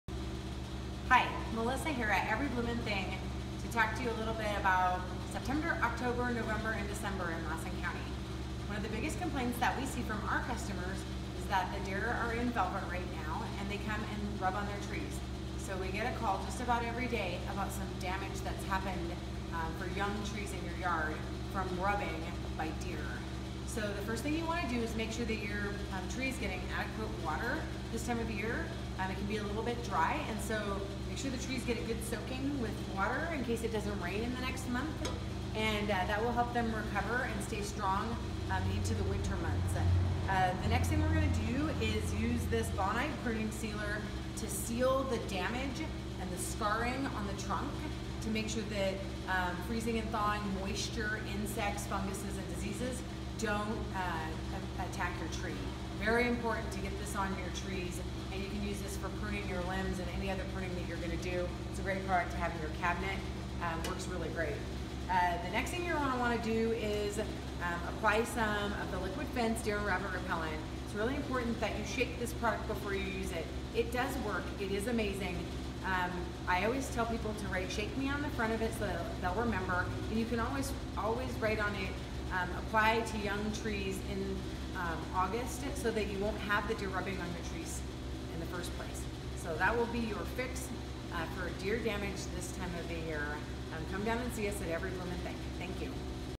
Keeping Deer out of the garden
Melissa describes some ways to keep deer out of the garden this coming winter.